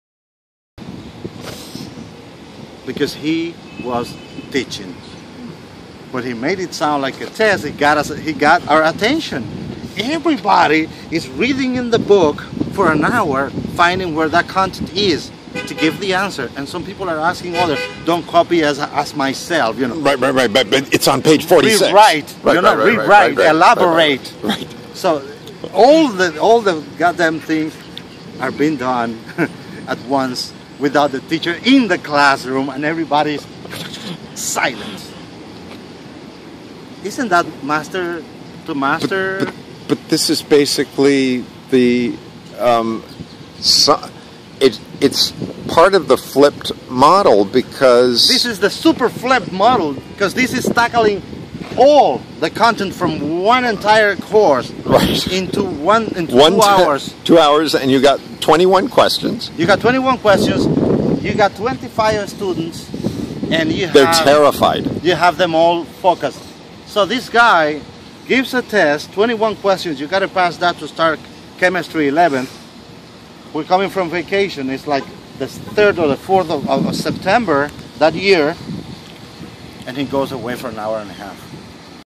Gustavo Fraga, an amazing teacher (who taught Mario J. Llorente) 2 of 4
Fraga taught chemistry in Cuba and knew interesting ways to get students to look up answers.  Here is Mario's story of the ultimate flipped classroom.